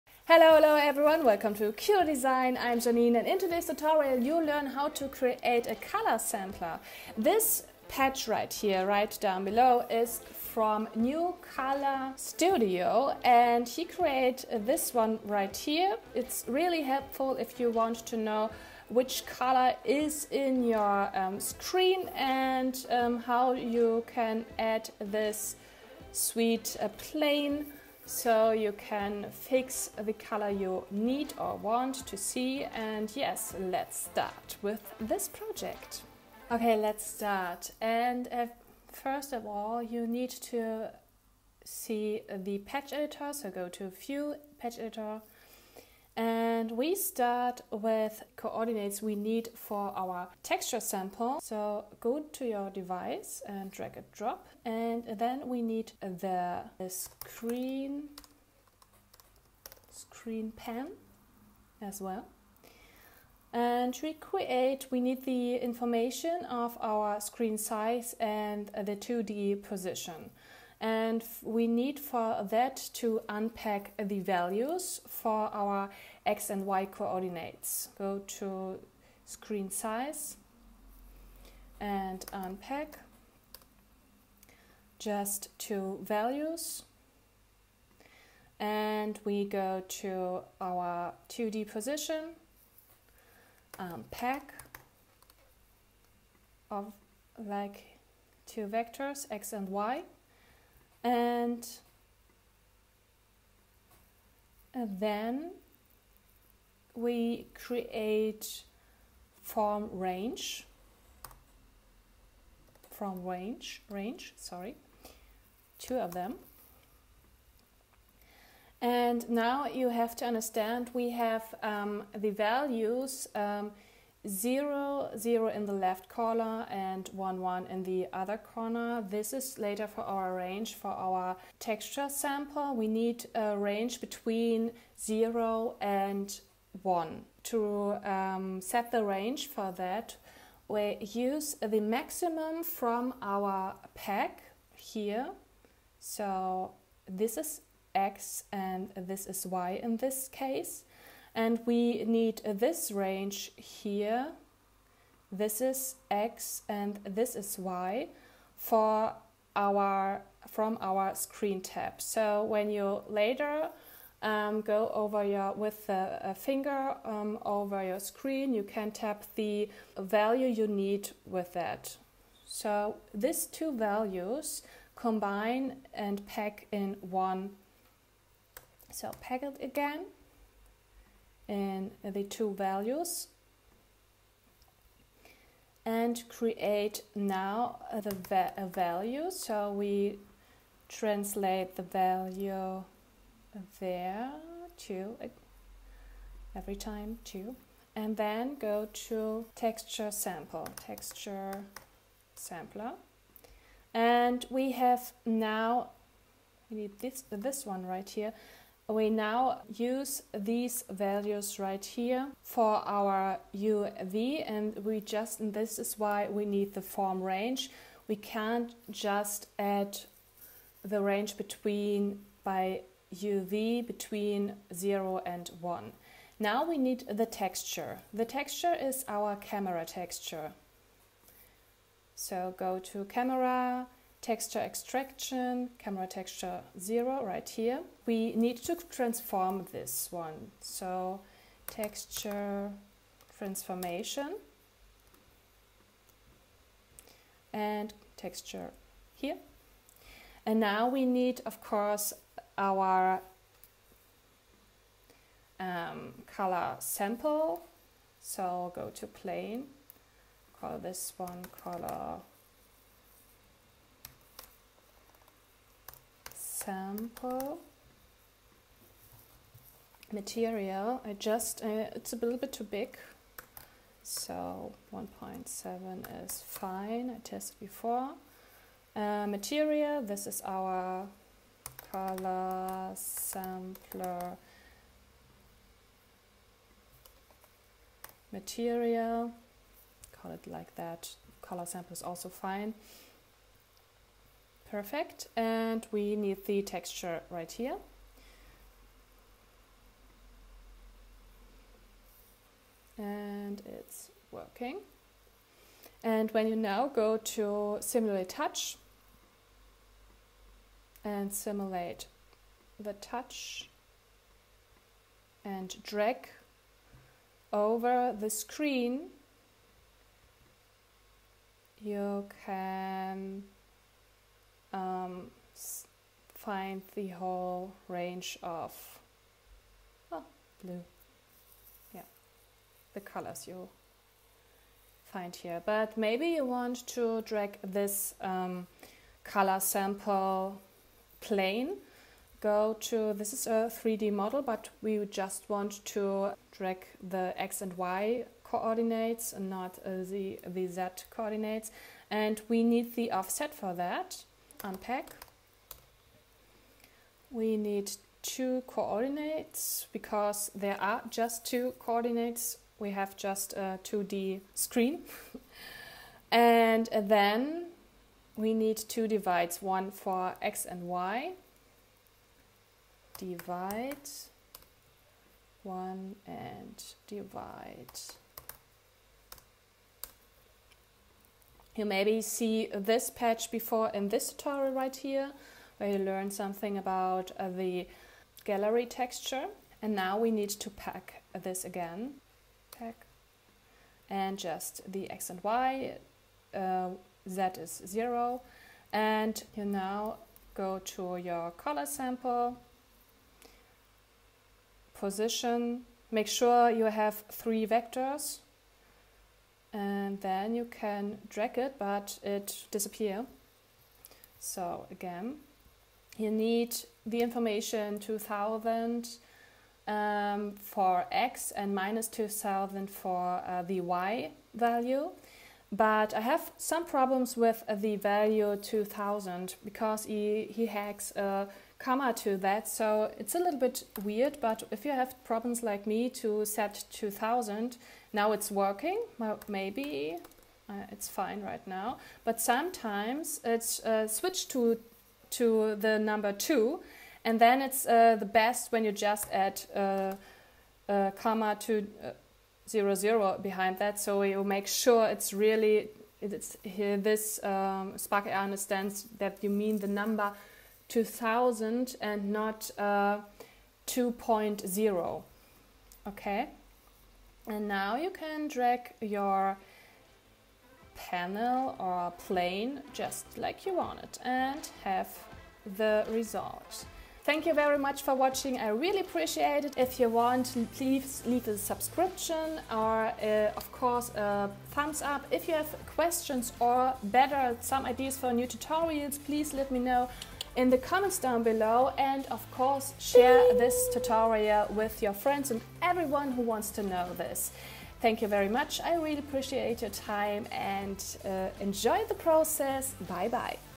How to create an Instagram Color Picker Filter in Spark AR
Learn in this tutorial to create a basic color sampler as Instagram filter in Spark AR. This tutorial is perfect if you want to separate specific colors from your video materials or photos. This is also a nice tool if you want to create a unique color palette by using your own designed story filter.

All credits and big thanks goes to @newcolorstudio, who created this patch asset.

0:00 Intro
0:36 Unpack the coordinates for the texture sampler
3:17 Add the color sampler
5:31 Add screen pan to the panel position

▬ EQUIPMENT ▬▬▬▬▬▬▬▬▬▬▬▬
Kamera: MacBookPro
Postproduktion: Premier CC + After Effects CC

QKnow Design wird produziert von Janine Konerow